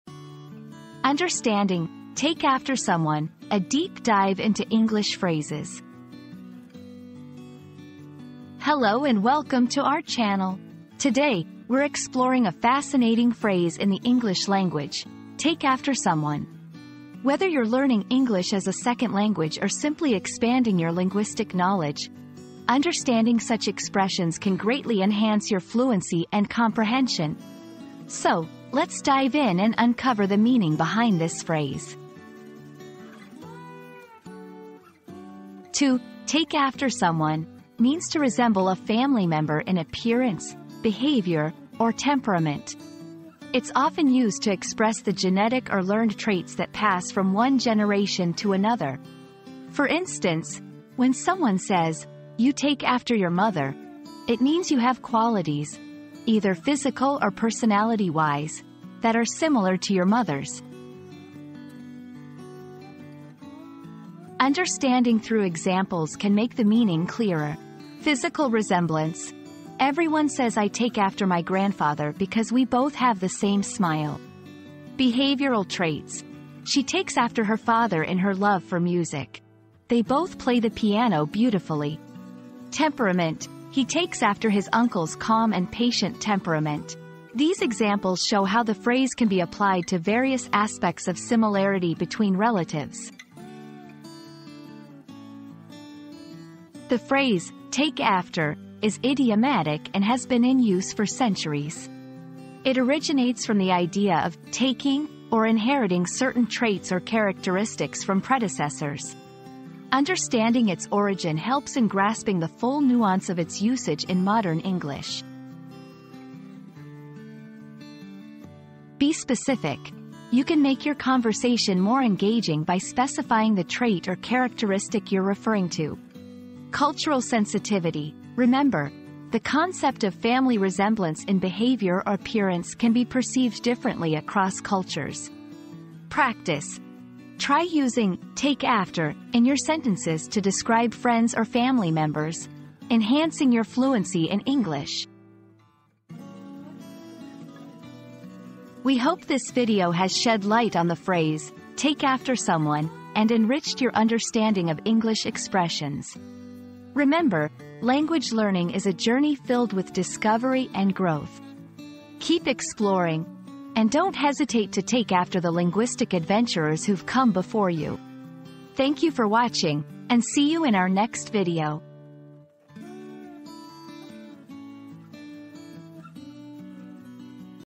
Understanding "Take After Someone": A Deep Dive into English Phrases
Unpacking 'Take After Someone': A Dive into English Phrases • Join us as we delve into the meaning and usage of the phrase 'take after someone' in English, exploring its origins and nuances.

00:00 • Introduction - Understanding "Take After Someone": A Deep Dive into English Phrases
00:35 • What Does "Take After Someone" Mean?
01:03 • Examples and Usage
01:36 • Origin of the Phrase
01:58 • Tips for Using "Take After Someone"